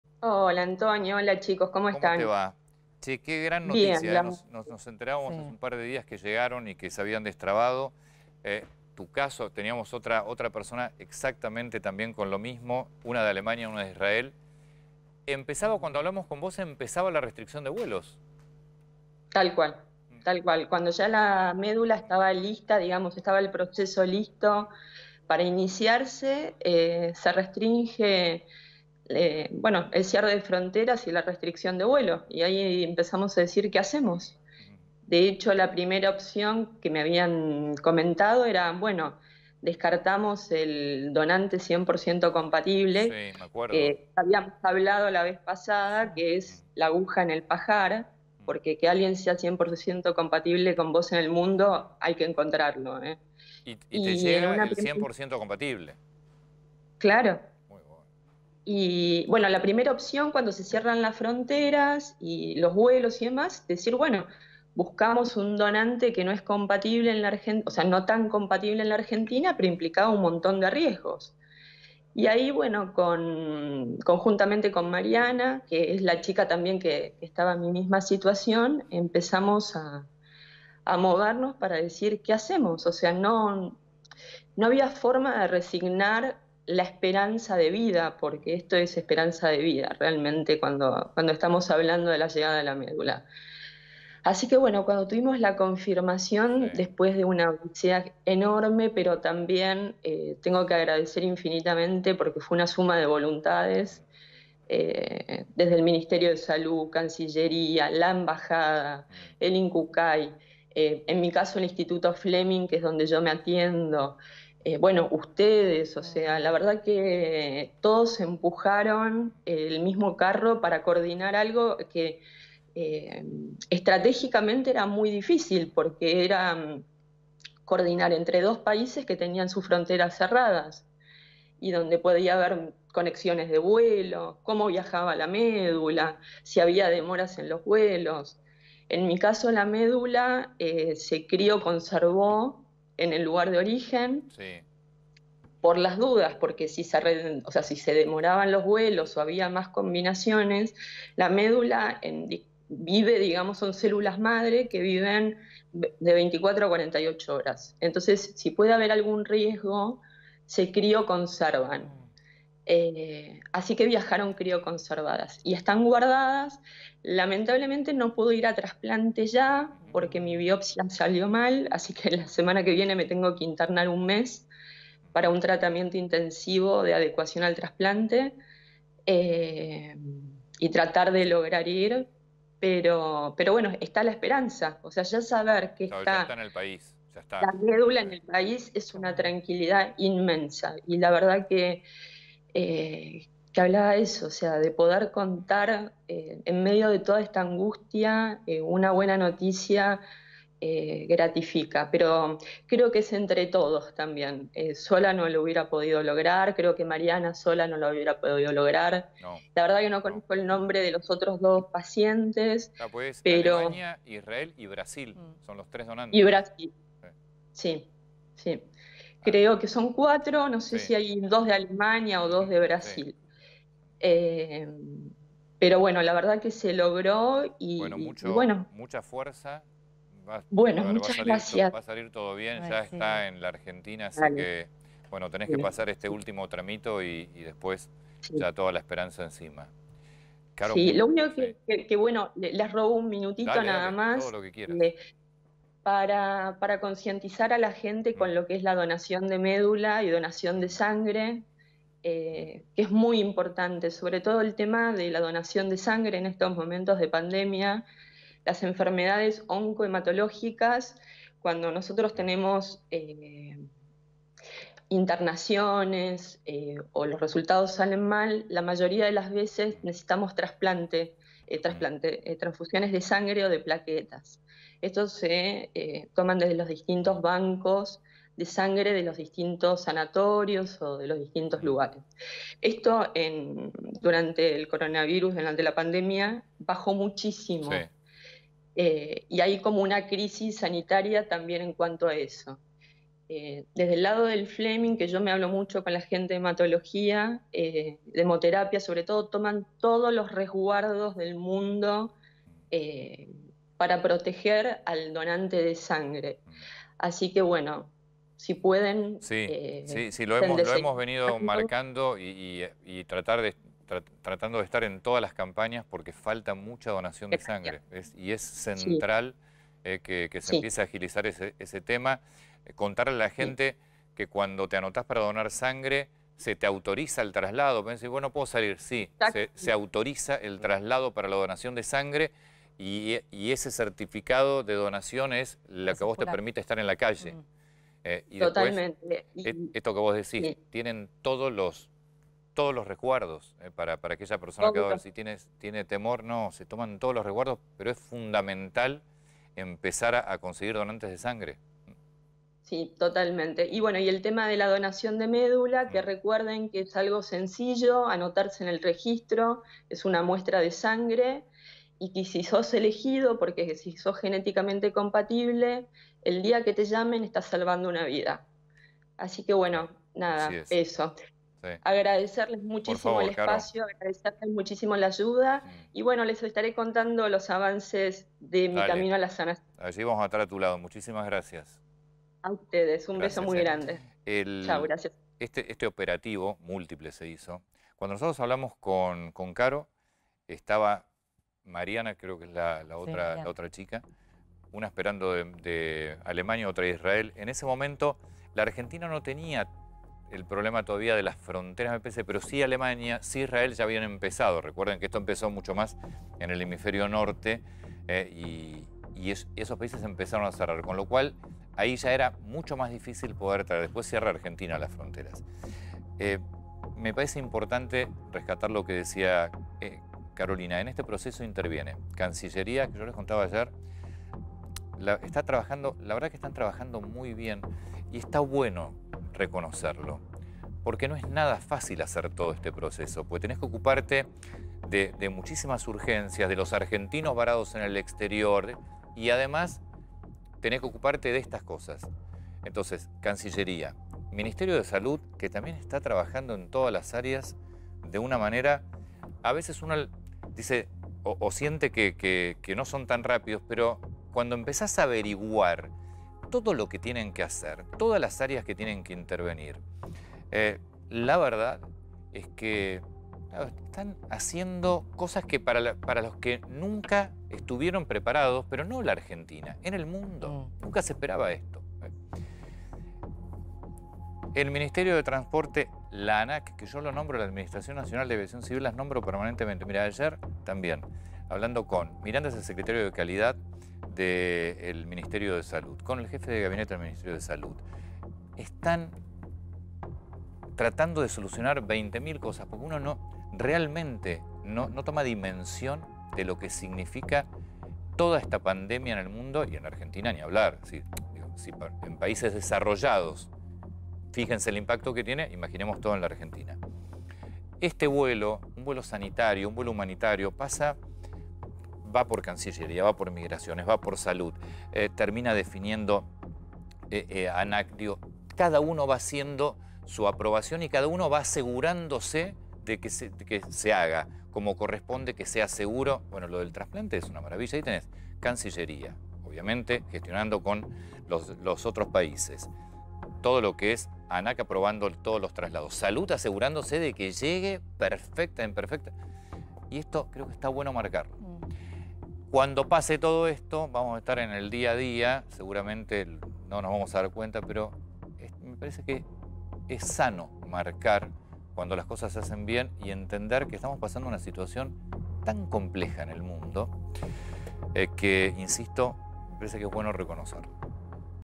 En un vuelo extraordinario llegaron trasplantes de médula ósea
En un vuelo extraordinario llegaron trasplantes de médula ósea, Carolina Embón cuenta que esto fue una "esperanza de vida". Habló en @BDAprograma por @A24COM